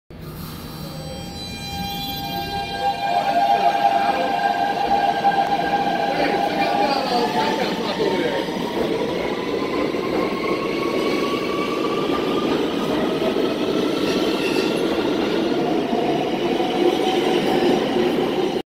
Brooklyn bound R160 on Siemens F local train leaving West 4 St
just taking a video of public transportation in NYC